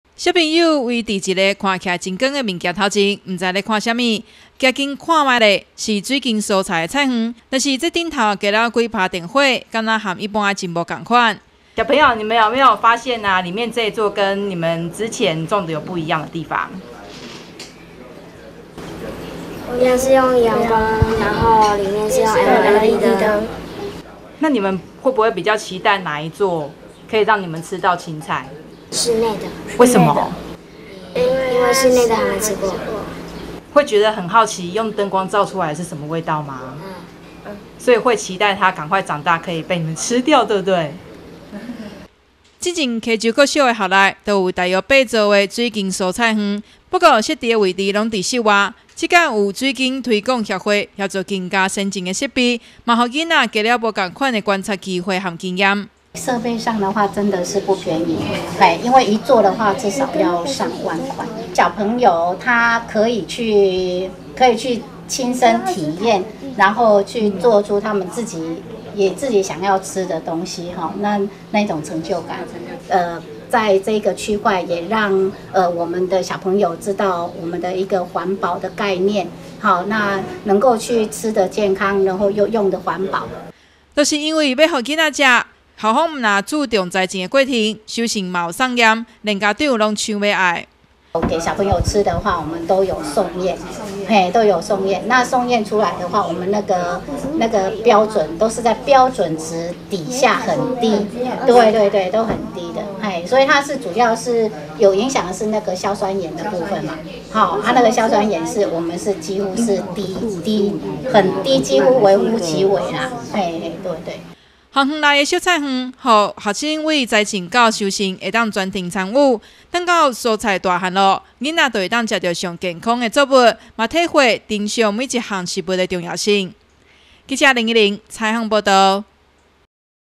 雲林新聞網-溪洲國小LED水耕蔬菜
種菜除了靠水和日照外，現在用節能的光電設備，也能讓蔬菜順利長大，水耕推廣協會贊助斗六市溪洲國小，設置了一座有LED照明設備的水耕蔬菜園，和一般的最不一樣的，就是菜園可以放在室內，不用怕風吹雨打，而且還能讓小朋友來觀察，在太陽照射下和LED燈下所栽種的蔬菜，是不是有不一樣的地方，特殊的設備，吸引了小朋友的注意，也成為校內最受注目的新設備。